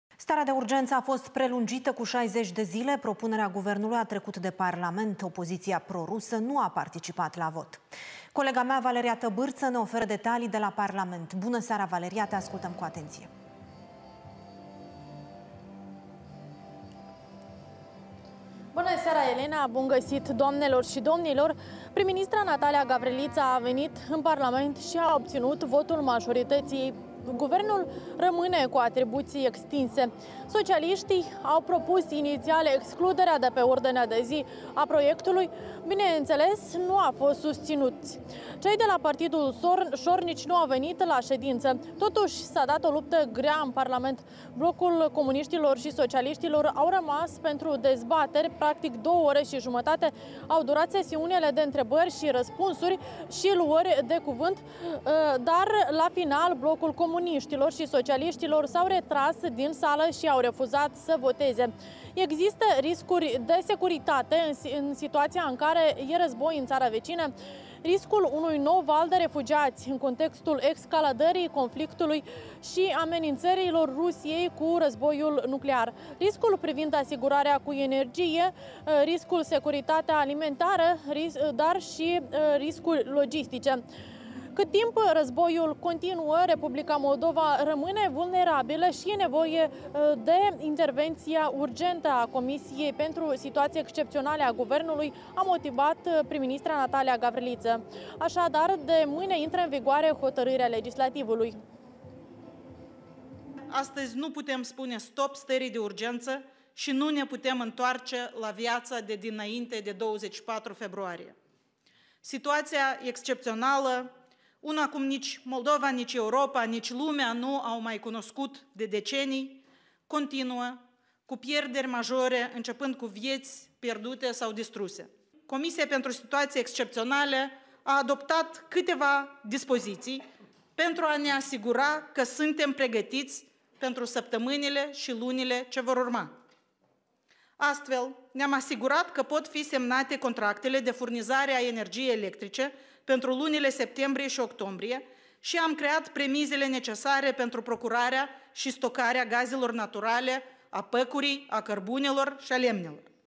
Starea de urgenţă în Republica Moldova a fost prelungită cu încă 60 de zile
În Republica Moldova a fost prelungită starea de urgenţă cu încă 60 de zile. Propunerea Guvernului a fost susţinută cu votul a 53 de deputaţi PAS. Deputaţii Blocului Comuniştilor şi Socialiştilor au părăsit sala de plen.